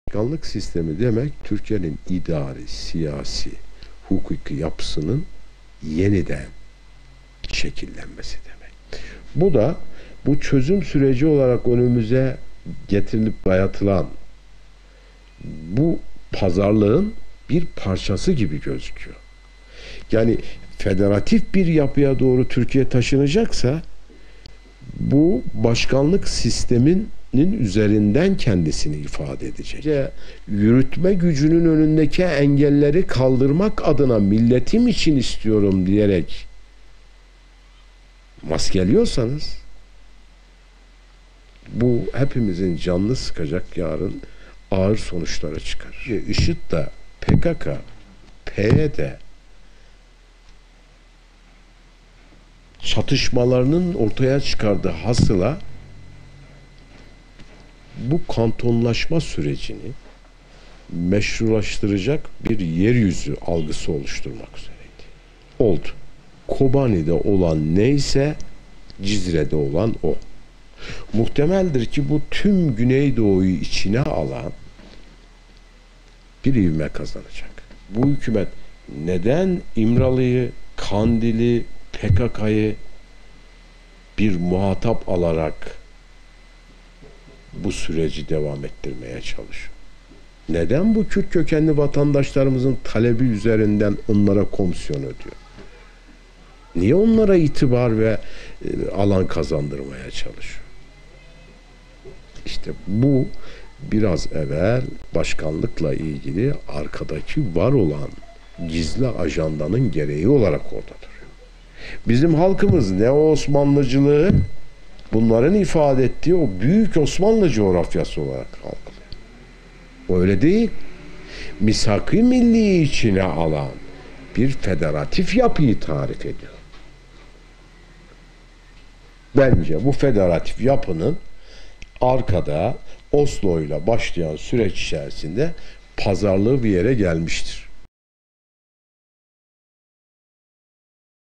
'BAŞKANLIK İDDİASININ ARKASINDA GİZLİ BİR AJANDA VAR'
Refahyol Hükümeti'nin Devlet Bakanlarından Gürcan Dağdaş Başkanlık Sistemi'yle ilgili önemli değerlendirmelerde bulundu. Başkanlık Sistemi'nin arkasındaki gizli ajandanın Federatif Yapı olduğunu söyleyen Dağdaş, bunun herkes için ağır sonuçlar doğuracağını söyledi.